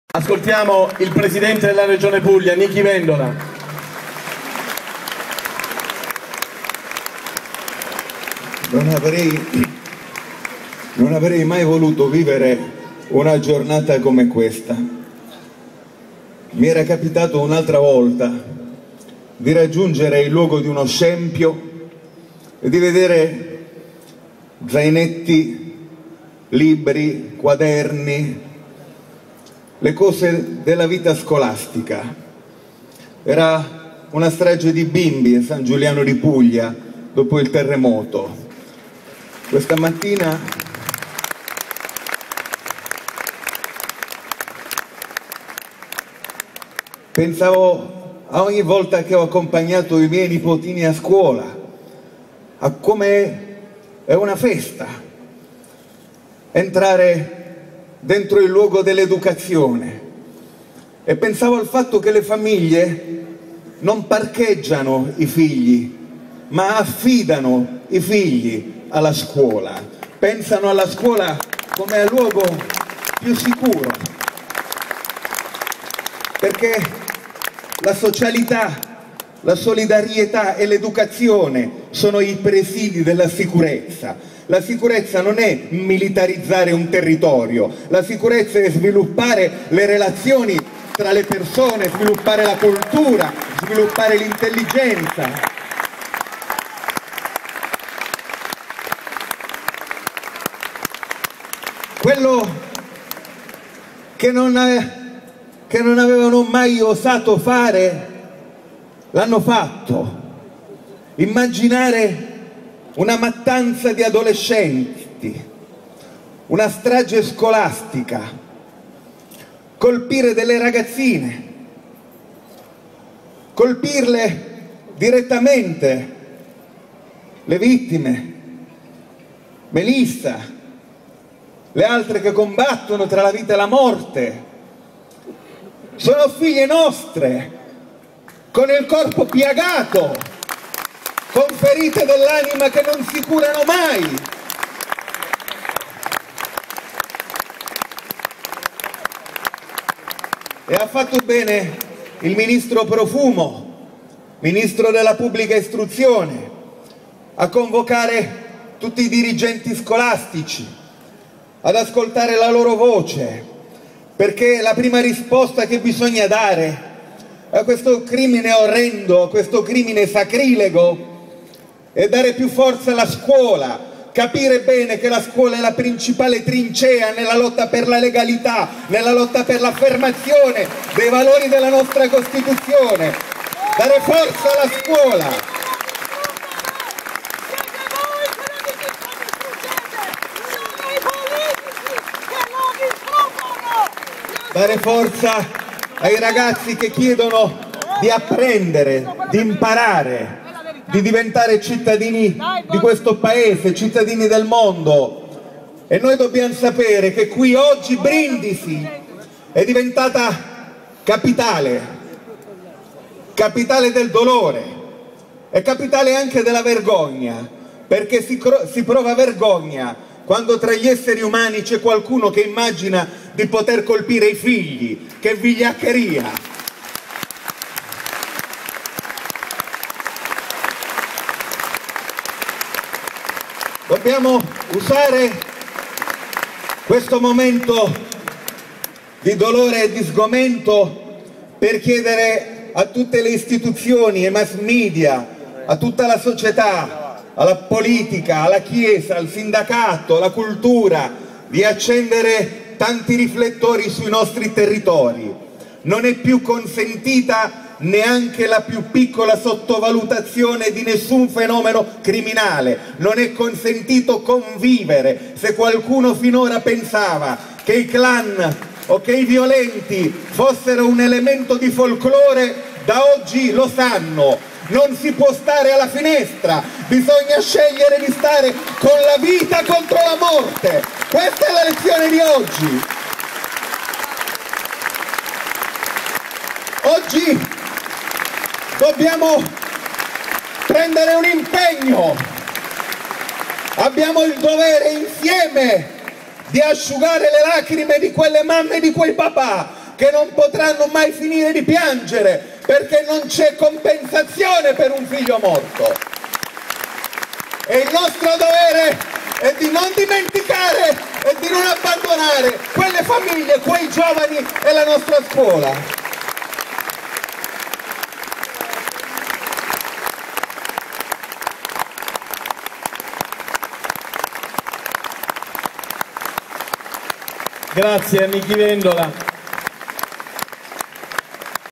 Vendola: "Lacrime eterne, non c'è compensazione per la morte di un figlio"
Sul palco di Brindisi, il Governatore della Regione Puglia, Nichi Vendola, è commosso: "Quei vigliacchi, autori di un crimine sacrilego, hanno colpito le nostre figlie, che ora hanno il corpo piagato e ferite all'anima che non guariranno mai". E conclude: "Dobbiamo asciugare le lacrime di quelle mamme e quei papà che non smetteranno mai più di piangere: per la morte di un figlio non c'è compensazione".